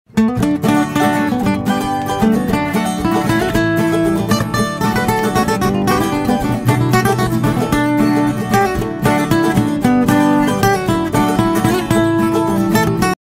TimeLapse webcam Monte Della Tesa 28 2 2018
Frazione di San Rufillo a Casola Valsenio (RA)  vista dalla webcam della stazione meteo Monte Della Tesa.